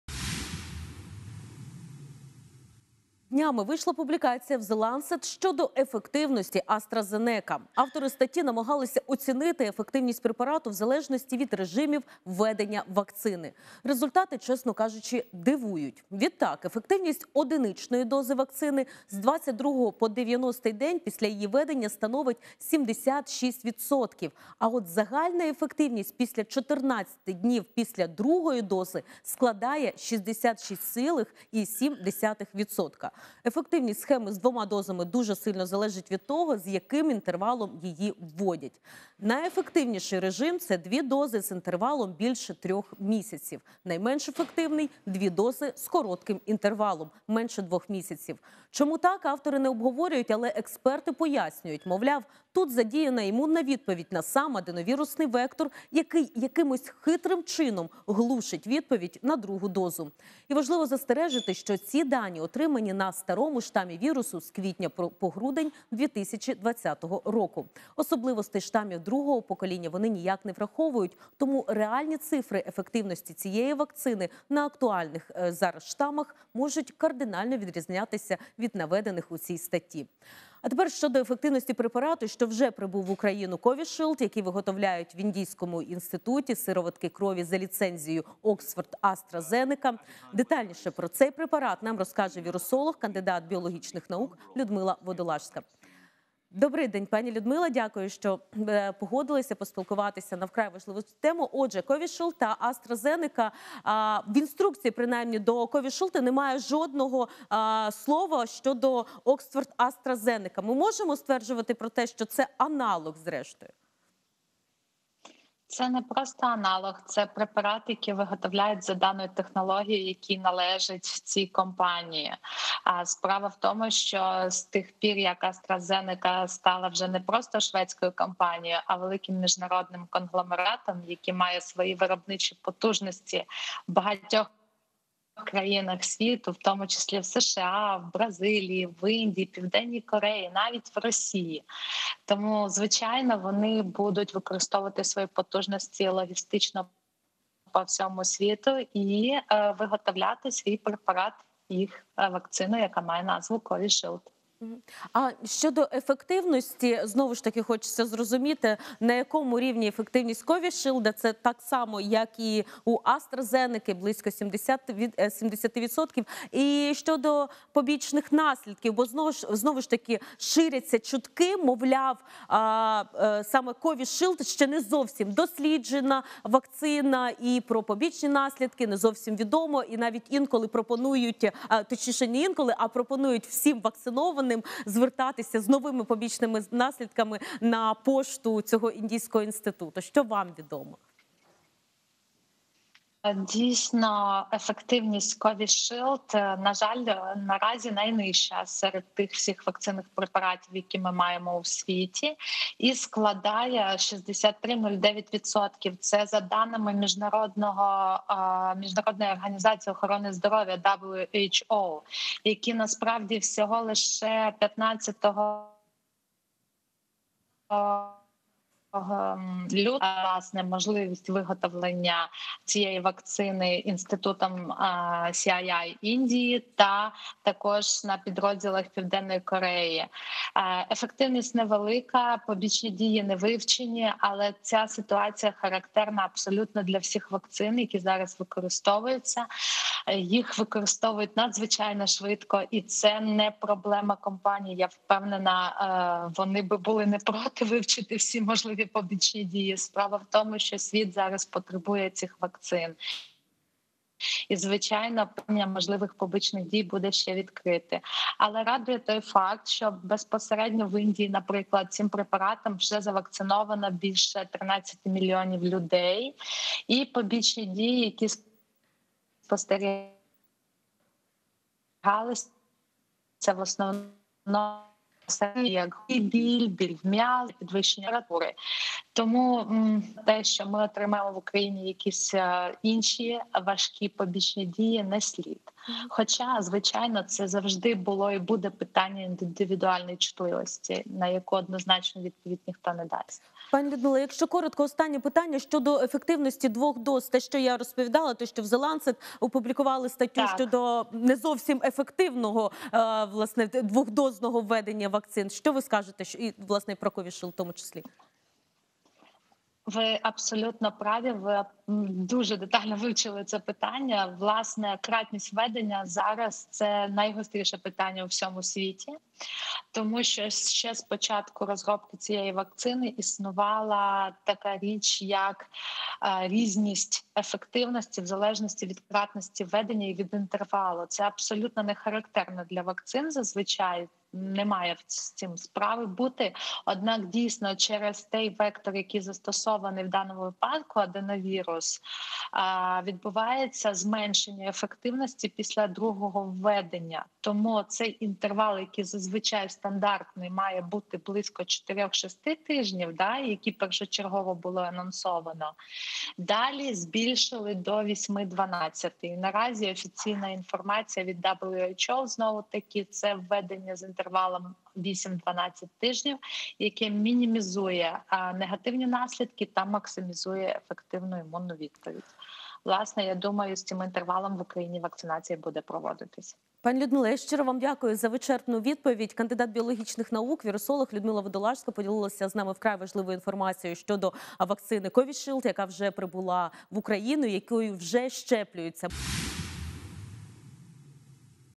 Наскільки ефективна індійська вакцина від коронавірусу Covishield?
Наскільки ефективна індійська вакцина від коронавірусу Covishield, яка фактично є аналогом препарату "Oxford-Astra Zeneca". Результати досліджень кажуть, що найбільшу ефективність дають  дві дози індійської вакцини з інтервалом більше 3 місяців. Найменш ефективний - дві дози з коротким інтервалом менше 2 місяців. Чому так - автори не обговорюють, але експерти пояснюють.  Мовляв, тут  задіяна імунна відповідь на сам аденовірусний вектор, який хитрим чином "глушить" відповідь на другу дозу. Важливо застережити, що ці дані отримані на "старому" штамі вірусу з квітня по грудень 2020 р., тобто йде мова про те, що вакцина Oxford-Astra Zeneca може бути неефективною в боротьбі з новими штамами коронавірусу.
Чи ефективний та безпечний препарат Covishield, який виготовляють в індійському Інституті сироватки крові за ліцензією Oxford-Astra Zeneca розповіла вірусолог, кандидат біологічних наук Людмила Водолажська.